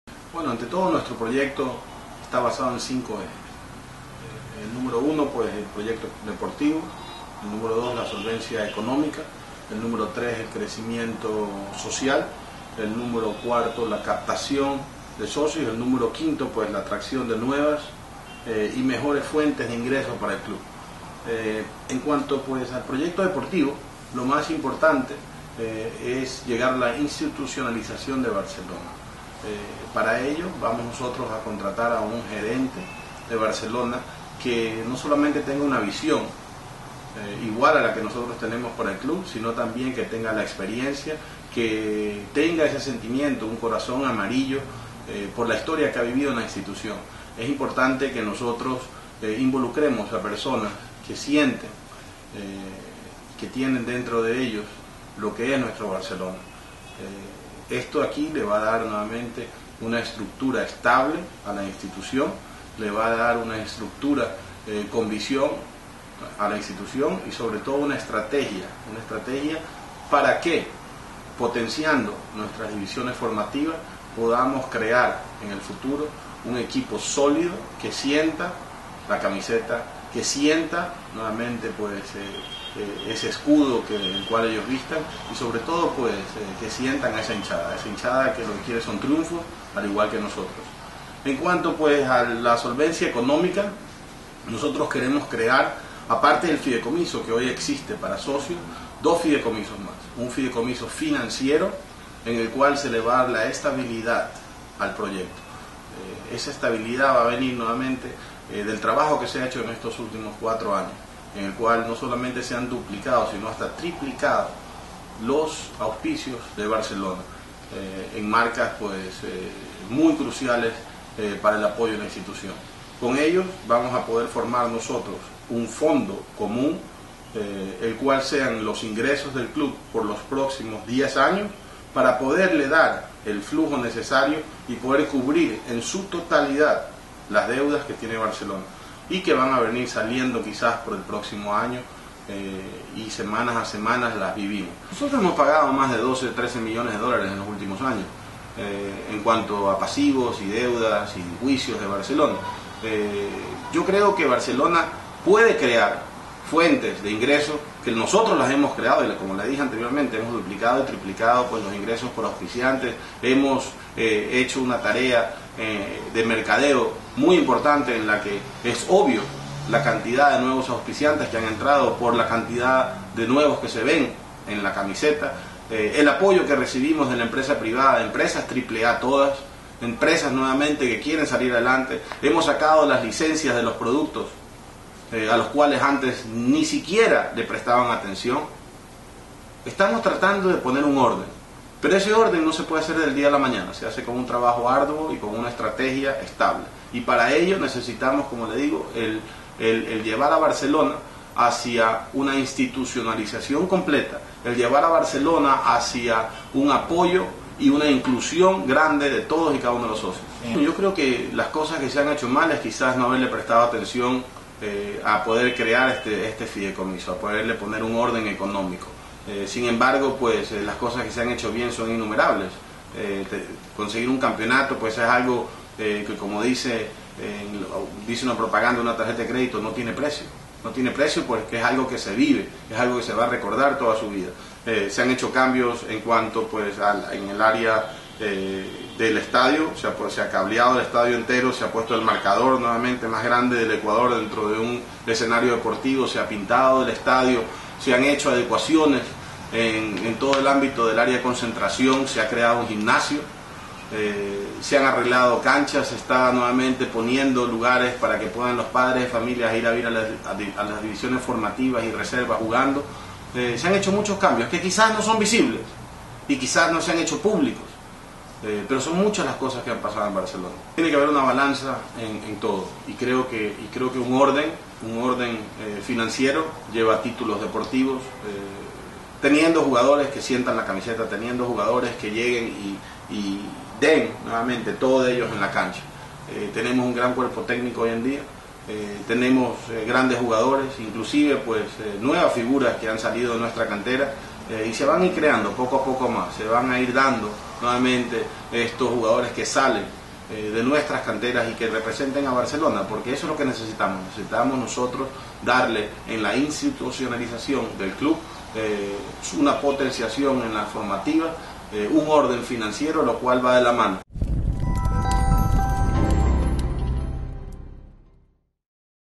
Luis Noboa: Los exjugadores no pueden presidir el club
La vida se vive de experiencias y las experiencias negativas o positivas son las que dan la pauta a qué llegar y qué hacer. Los grandes equipos del mundo son manejados por empresarios, no solamente por su visión empresarial o por la preparación sino por la credibilidad financiera que han mantenido.